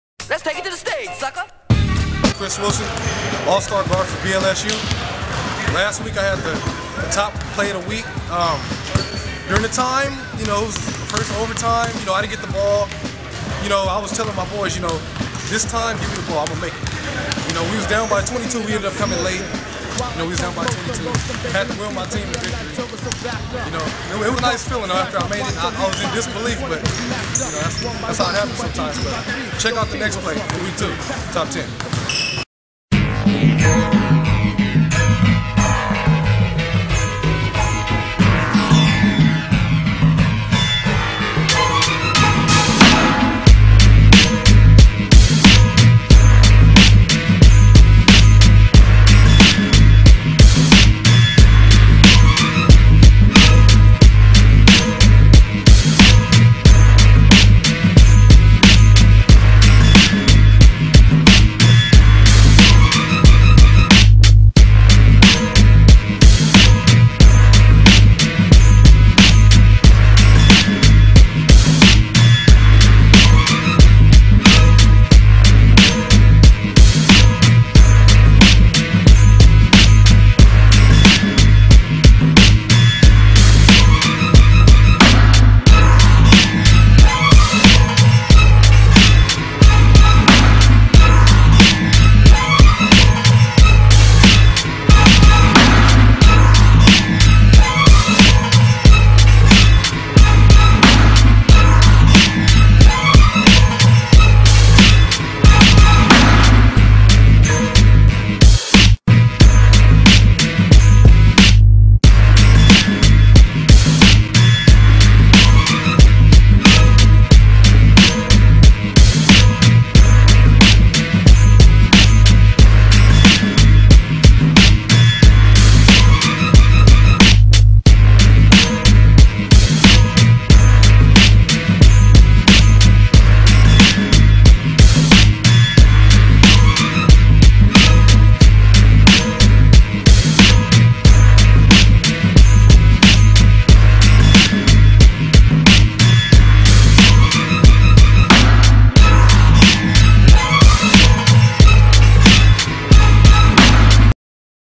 Spring 2011 - Week 2 Top 10 Highlights
Top 10 Highlights featuring Alphonso White, Larry Parker, Mike Enriquez, Chris Epps, Sheldon Pace, Jason McClure, Monica Hang.